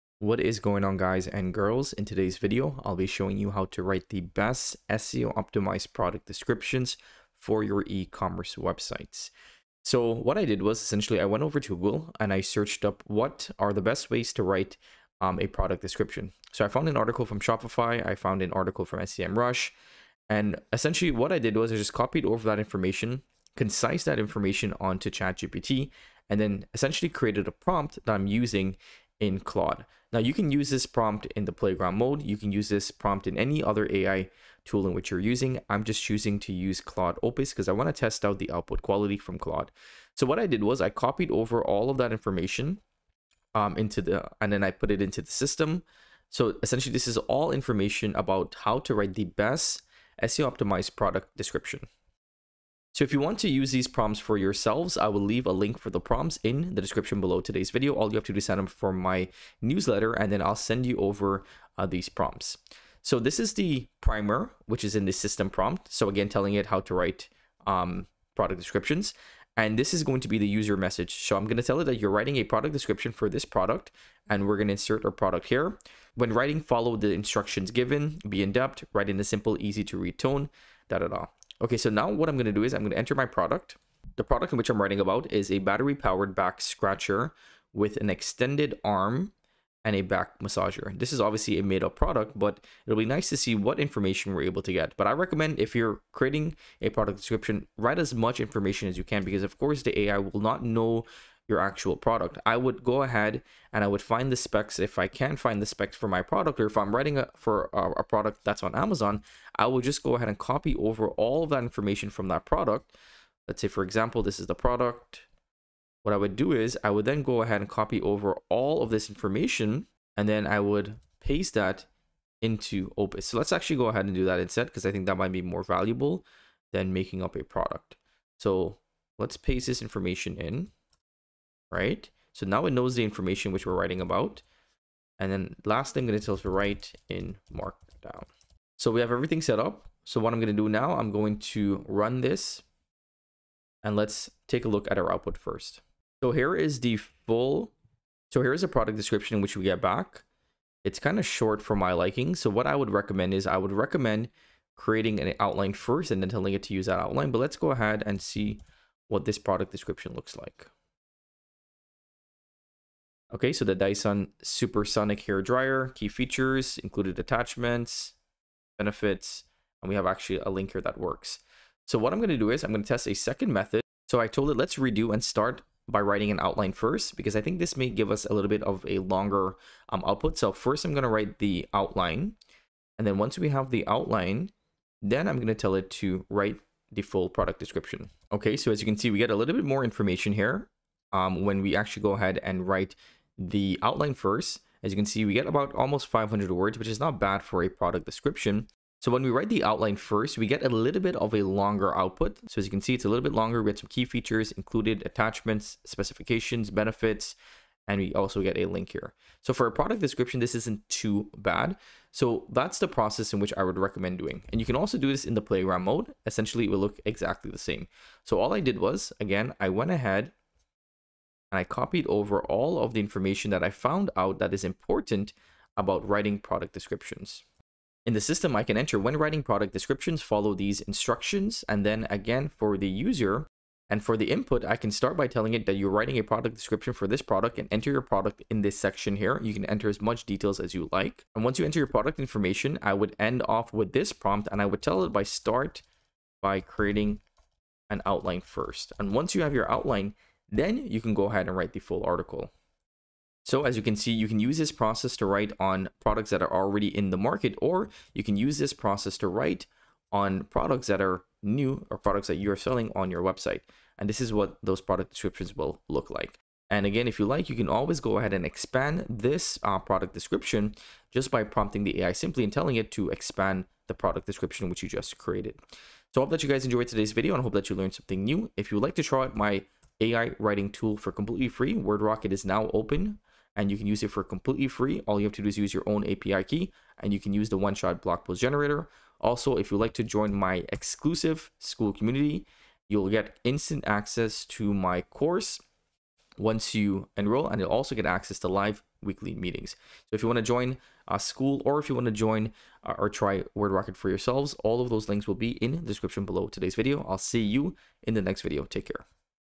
Secret AI Method To Write SEO Product Descriptions That Convert 🔥
Uncover the Secret AI Method to write SEO product descriptions that convert!

In this must-watch video, we reveal how AI can elevate your product content, driving more sales and boosting your search engine rankings. 🚀

🤖 Dive into the powerful capabilities of AI tools like Claude Opus and ChatGPT, and learn to craft product descriptions that are not only engaging but also highly effective at converting visitors into customers.

✅ What you'll learn:

The essentials of SEO-focused product descriptions.
A step-by-step guide on leveraging Claude Opus and ChatGPT for captivating descriptions.
Choosing the right AI prompts to generate descriptions that lead to sales.
Practical tips to enhance your descriptions for maximum impact.
📈 Perfect for entrepreneurs, marketers, and ecommerce professionals, this video delivers actionable insights to transform your product listings with AI. Boost your online presence and watch your conversion rates soar!

👉 Discover the secret techniques that will differentiate your products in the digital marketplace. Watch now and revolutionize your product descriptions with AI!